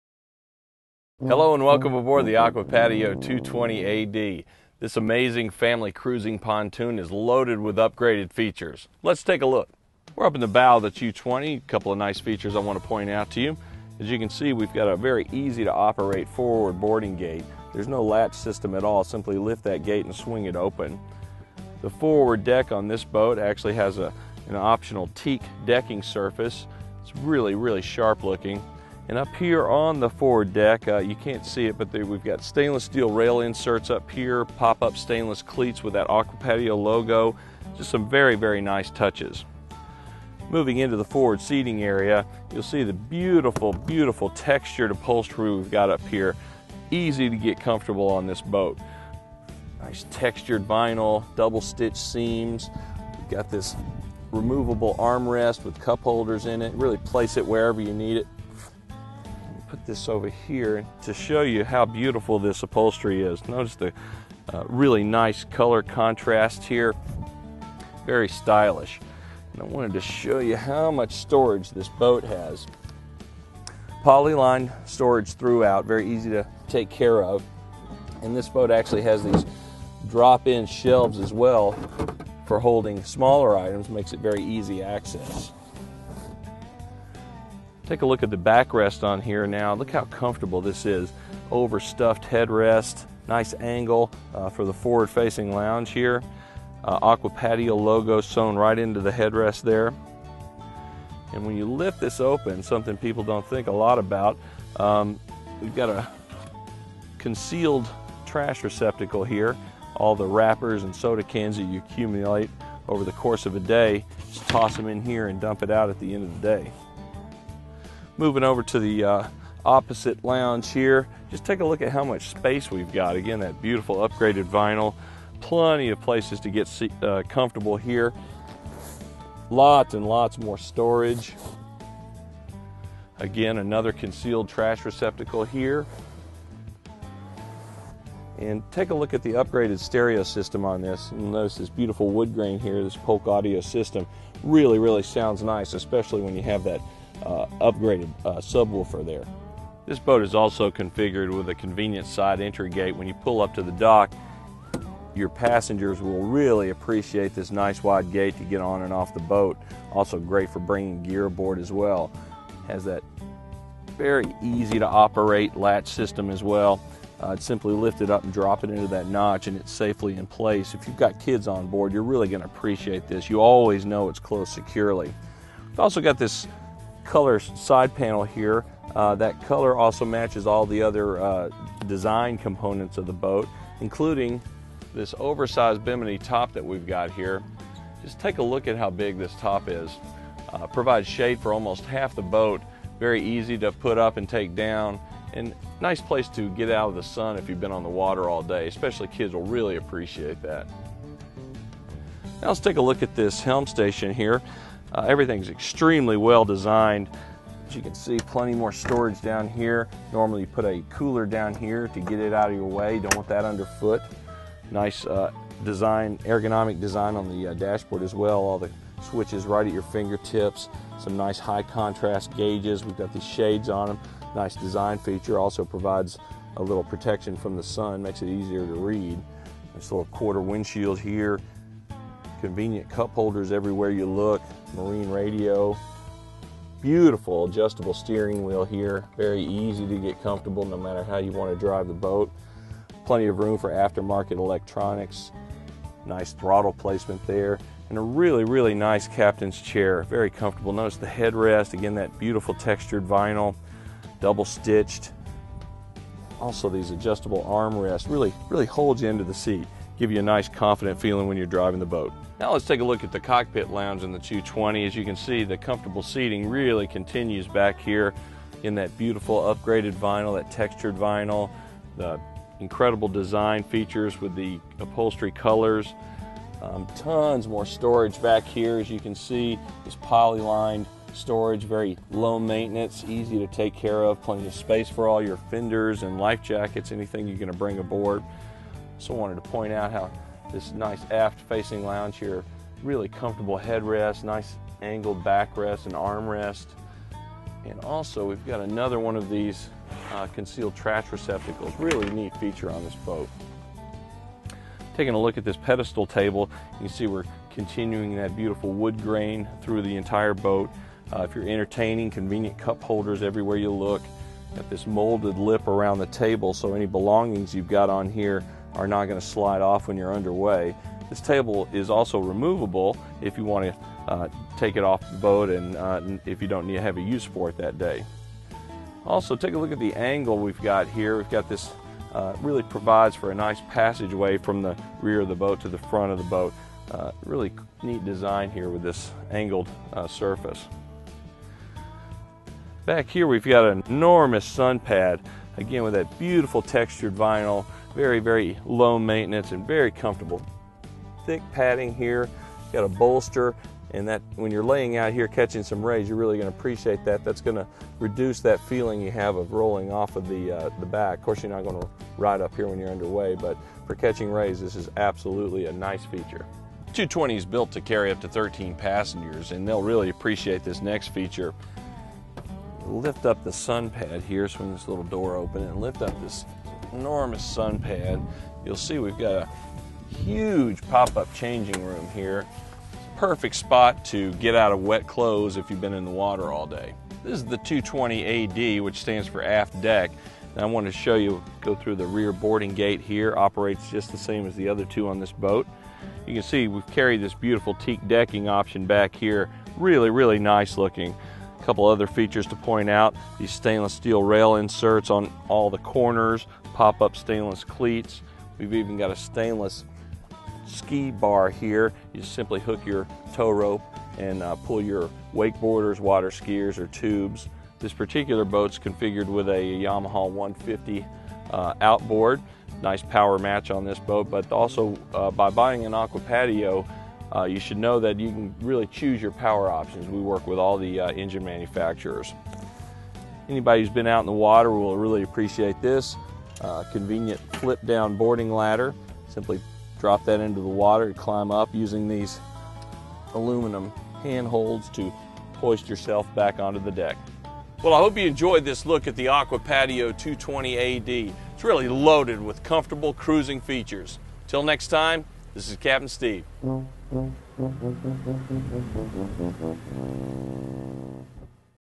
Điều đặc biệt trong Godfrey Aqua Patio 220 | SYMC
Chúng a cùng đến với chuyên gia của Godfrey để khám phá bên trong chiếc Aqua Patio 220 nhé
Saigon Yacht and Marina JSC (SYMC) | Dream – Explore – Memories Built
Add : 180c Hai Bà Trưng, Quận 1, TP.HCM